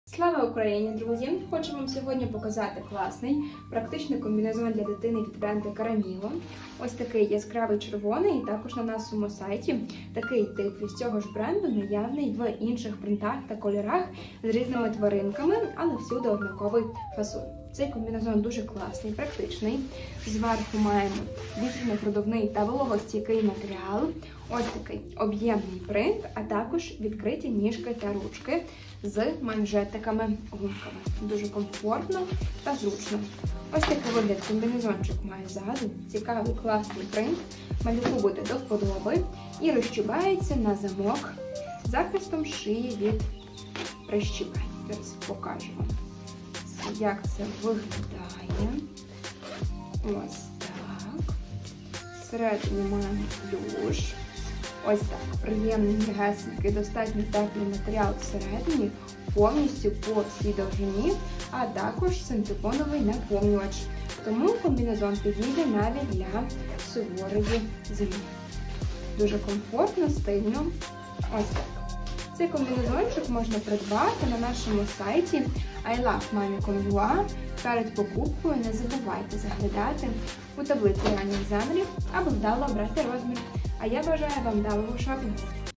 Комбінезон з плюшевою підкладкою для дитини від Сaramio
Замовити комбінезон з плюшевою підкладкою для дитини від Сaramio ви можете в інтернет-магазині брендового дитячого одягу